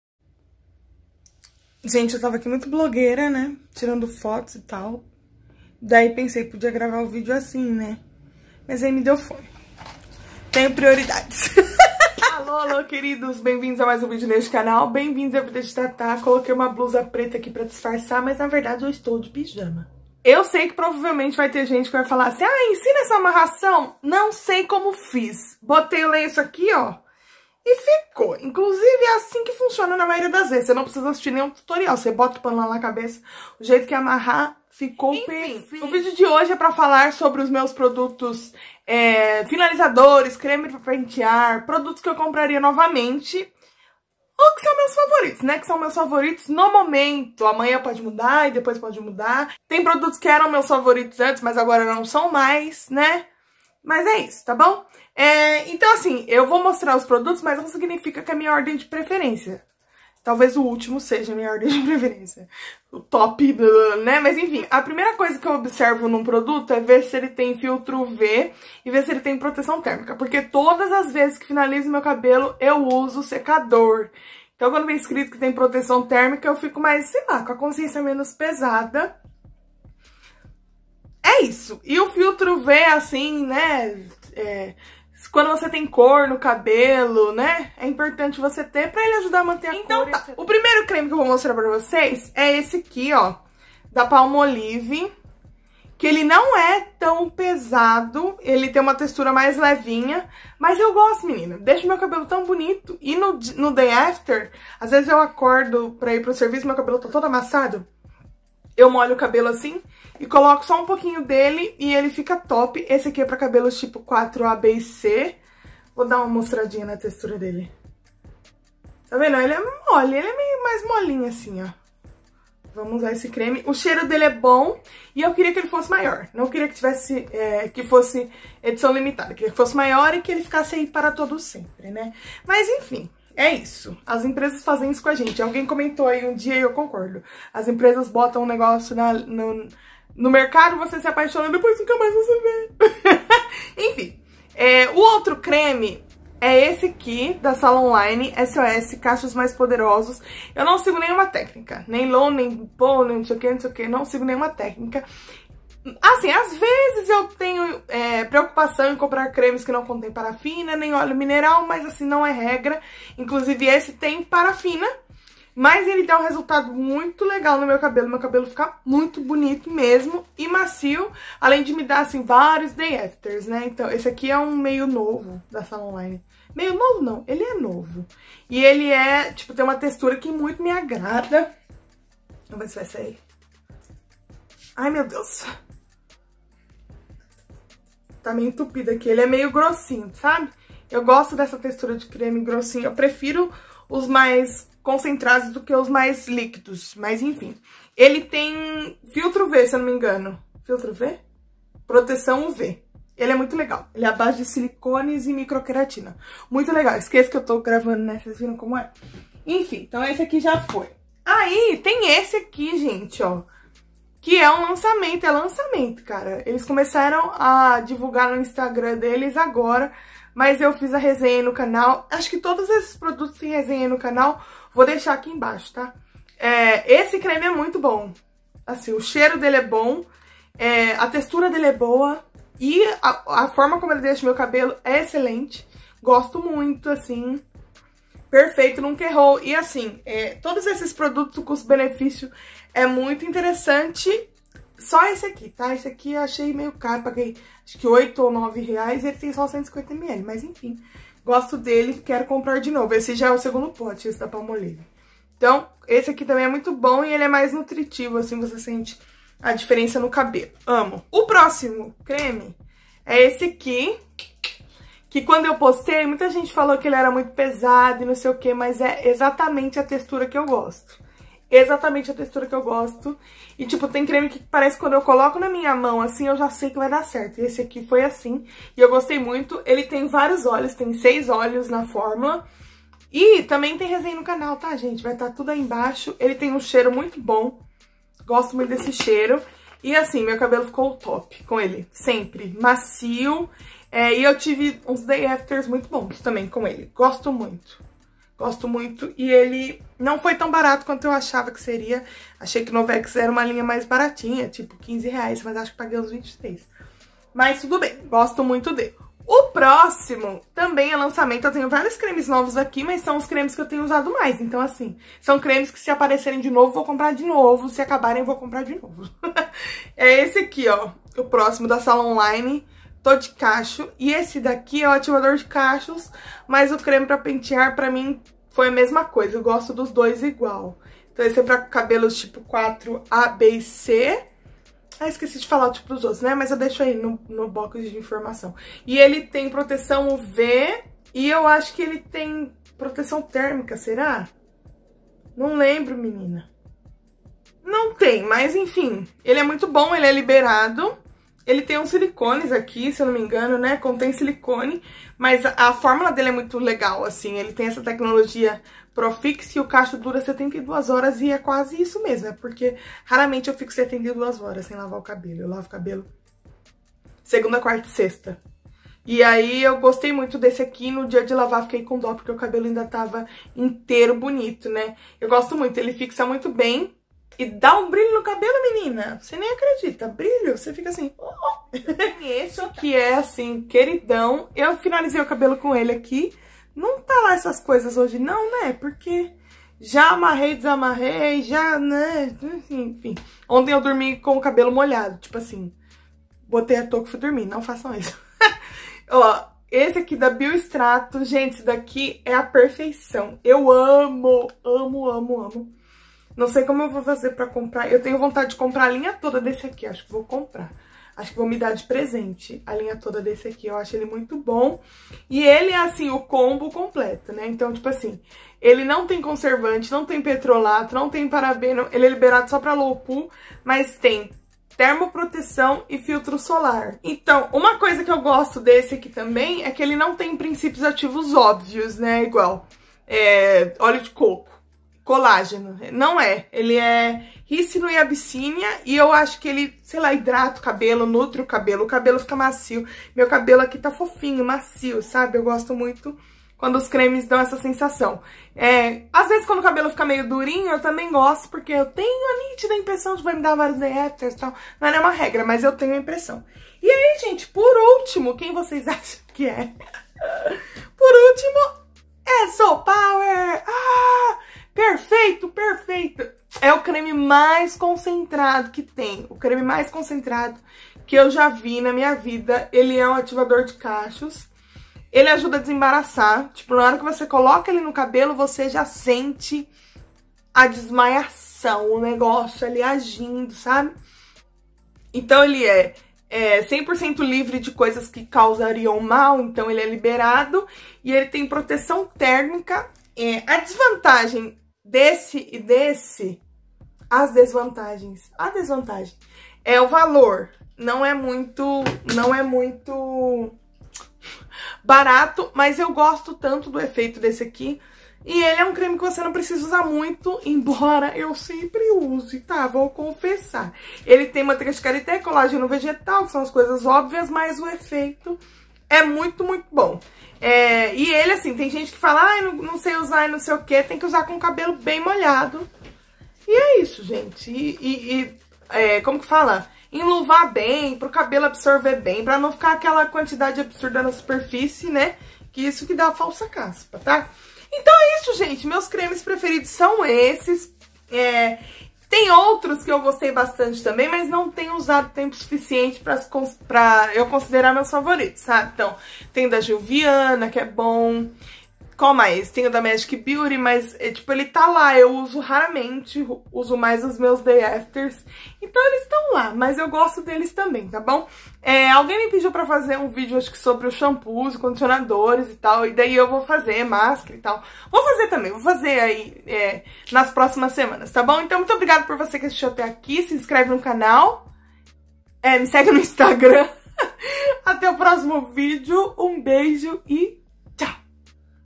MEUS CREMES DE PENTEAR E FINALIZADORES FAVORITOS- PRODUTOS QUE COMPRAREI NOVAMENTE!
oi gente!
no vídeo de hoje compartilhei meus produtos favoritos pra finalizar o cabelo!
na playlist abaixo tem as resenhas completinhas de todos eles.